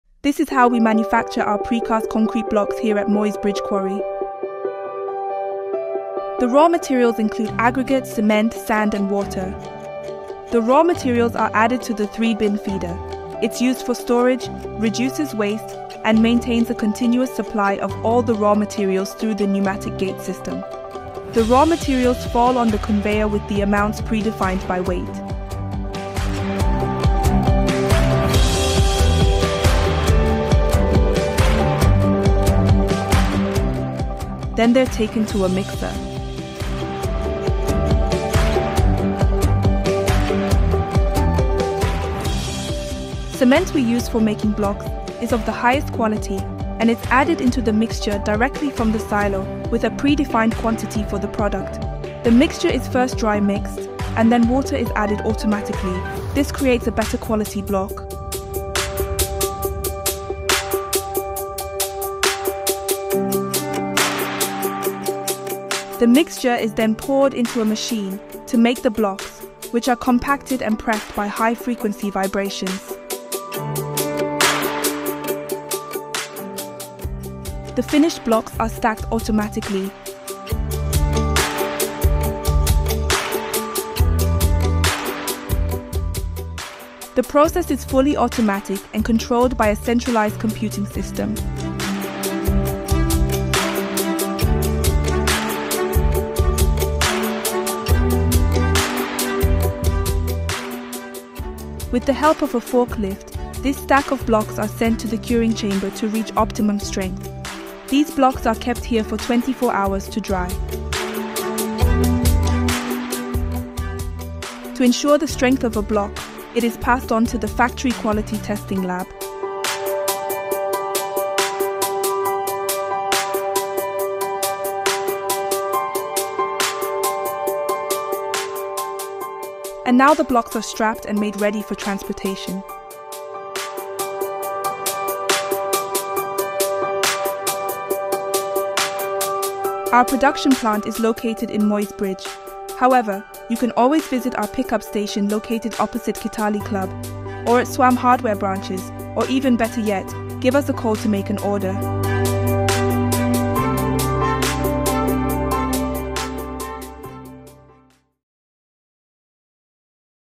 Hawen QT10-15 block making plant in Kenya,East Africa@hawenmachinery
Project Location of Hawen QT10-15 block making plant in Kenya: Moi’s Bridge,Kenya

HAWEN QT10-15 Automatic block making machine is one of the most popular Automatic block making machine in the market. It is a multifunctional block making machine,allows to make the hollow block,stock brick and maxie brick,interlocking paving stone,curb stone by changing the mould only.

Pallet size of machine: 1100x950mm or 1150x950mm

 Capacity for QT10-15:
- Hollow block /Solid block 390x140x190mm:
 14pcs/mould(for pallet size at 1150x950mm), approx.20,000~21,000pcs/8hours;

- Hollow block/Solid block 390x190x190mm:
10pcs/mould,approx.14000~15000pcs/8hours.

- Stock brick 200x100x70mm
52pcs/mould,10000pcs/8hours  per shift

- Maxie brick 290x90x140mm
30pcs/mould,57000pcs/8hours  per shift

- Quad Cabro(Rectangular Paver) 200x100x60mm:
 36pcs/mould,52000pcs/8hours,approx.1000 square meters per shift;
- Interlocking  paver 200x100x60mm:
 36pcs/mould,52000pcs/8hours,approx.1000 square meters per shift;
- Interlocking paver 225x112.5x60mm/80mm:
28pcs/mould,40300pcs/8hours,approx. 1000 square meter per shift ;

The control system of brick making machine is equipped with SIEMENS PLC and SIEMENS Touch Panel,world-famous  brand with quality assured.With  malfunction diagnosis system and remote control function,easy for operation and maintenance.

The Hydraulic system With“YUKEN” brand double proportional valve and directional valves, the flow and pressure of each movement could be adjusted on touch panel,machine running smoothly without impact,through the double vane pump,different actions run simultaneously,greatly improve production efficiency.
Main forming machine equipped with a basic Face-mix device,it can make the double layer paving stone just by changing the mould.